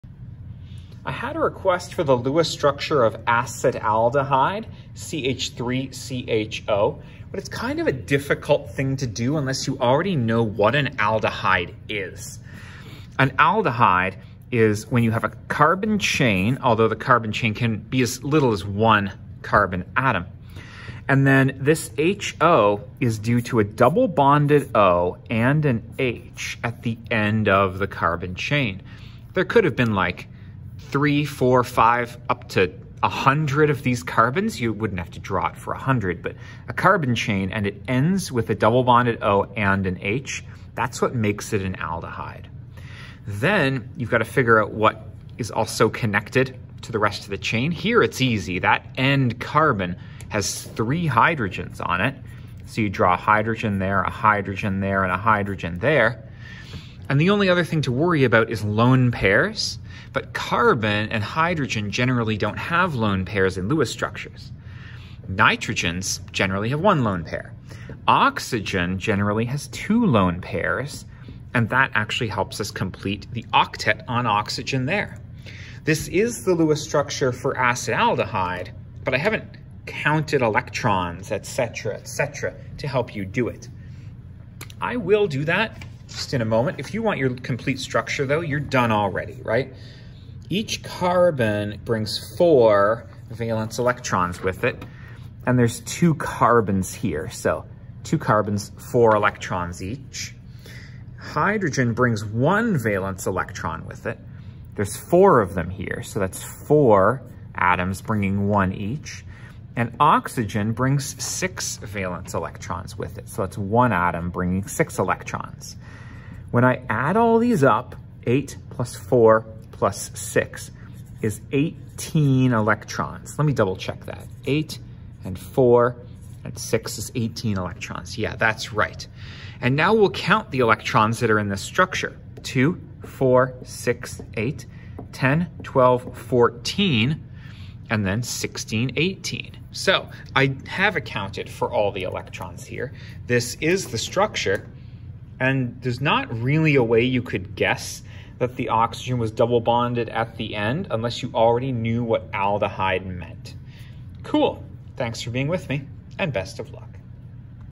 Lewis Structure of Acetaldehyde (CH3CHO)
Honestly, you have to KNOW what the structure of an Aldehyde is before drawing this Lewis Structure. There's no other way to know that the Oxygen is double-bonded to one of the carbon atoms.